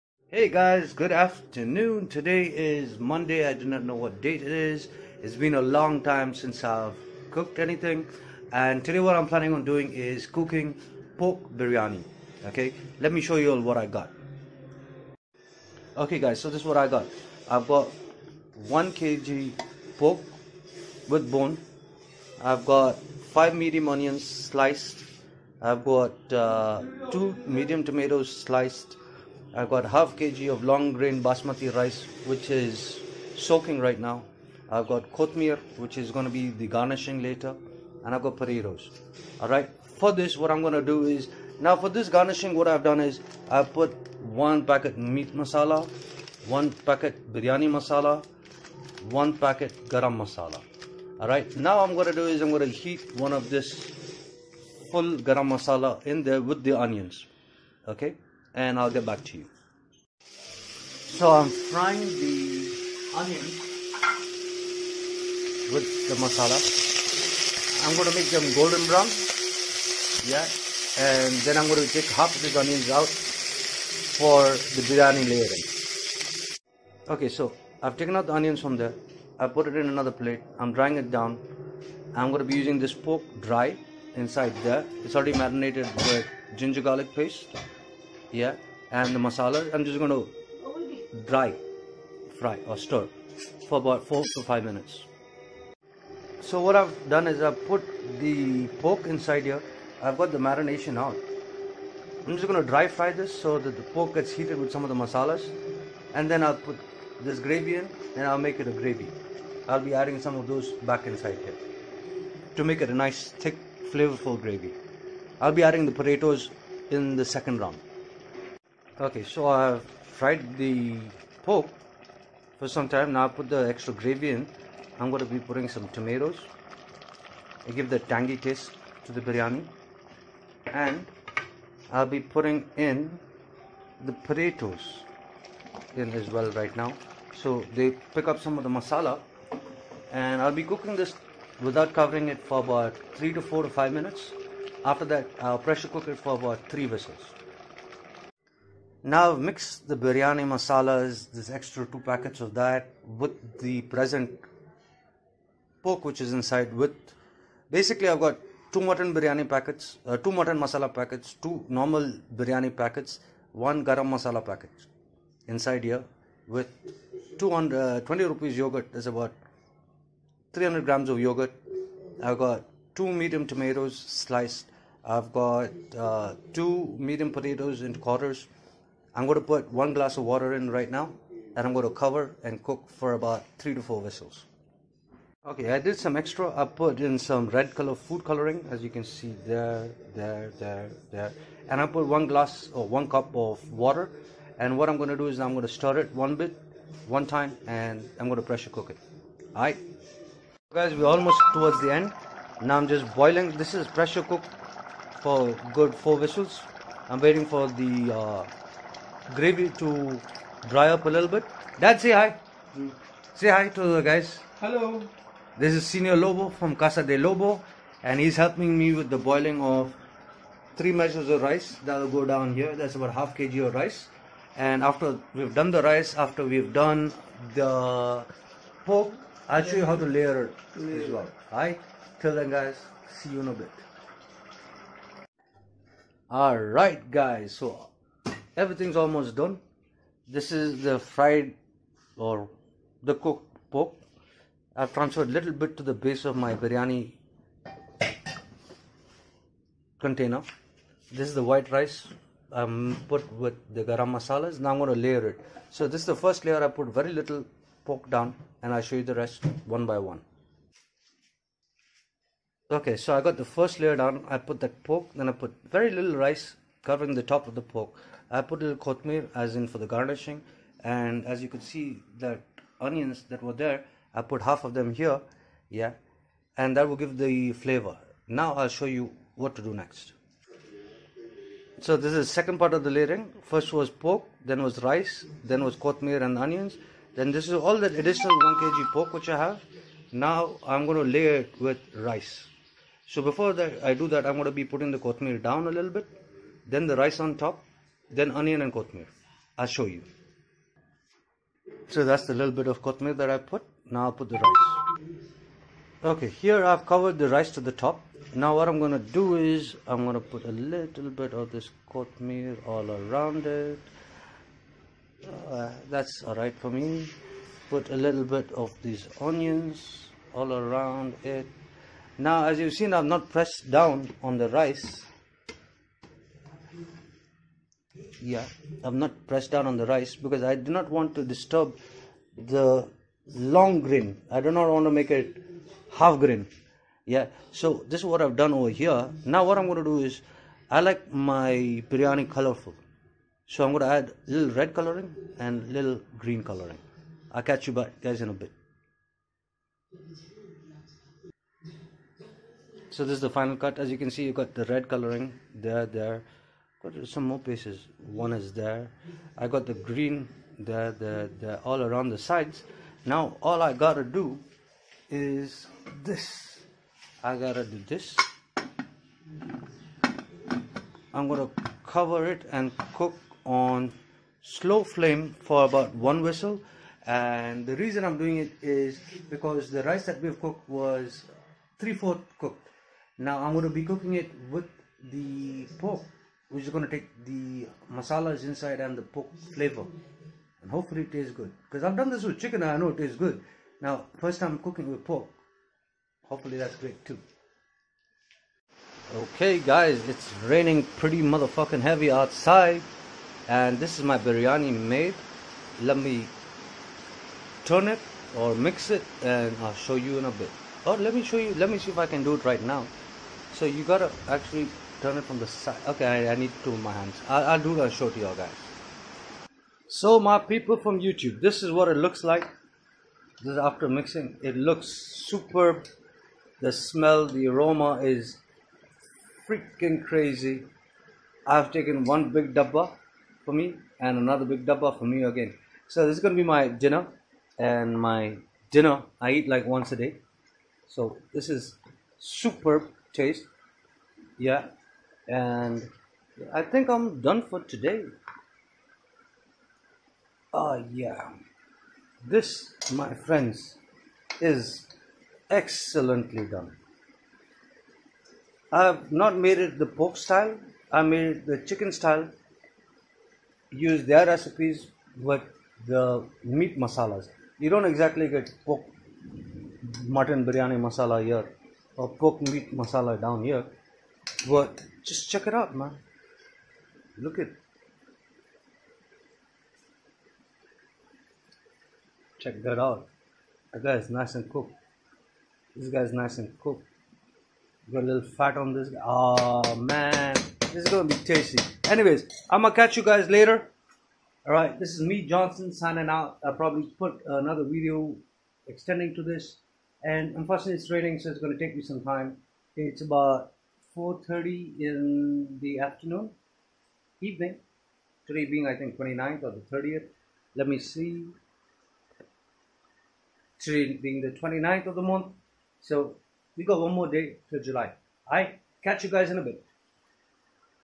Pork Byrani by Casa De Lobo Johnson
Pork byrani

Ingredients

Pork with bone.                 1kg
Basmati rice.                     .5kg
Onions medium.                5
Tomatoes medium.           2
Potatoes medium.             2
Byrani masala packets.    3(5rs)
Garam masala packets.   2(5rs)
Ginger garlic paste.           2 tbl spoon
Salt to taste
Chillie powder.                   2 tbl spoon
Coriander leaves.              Small bunch
Red and green colouring (not necessary)

Method of how to cook in the video..